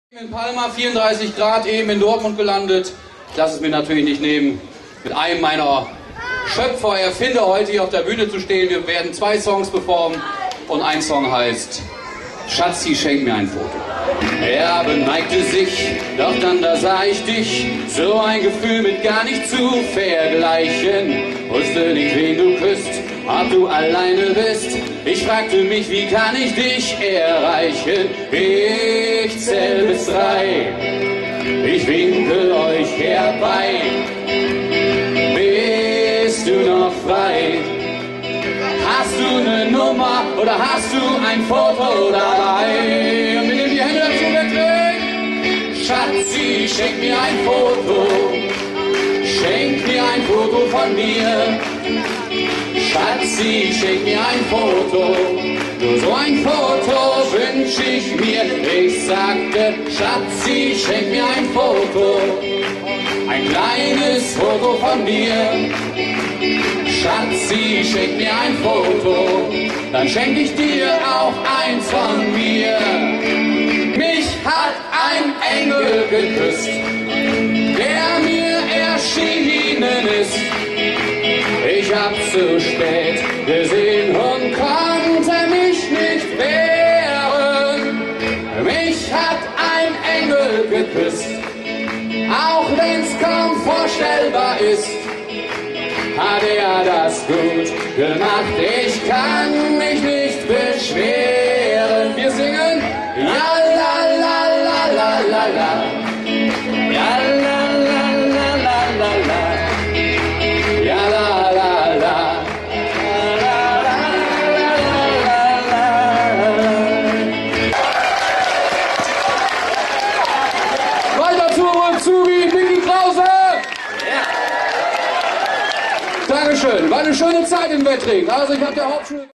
Wiedersehen mit Mickie Krause
30 Jahre nach der Schulentlassung der erste gemeinsame Auftritt: die Ballermann-Ikone MICKIE KRAUSE mit seinem Musiklehrer Walter Zurbrügg!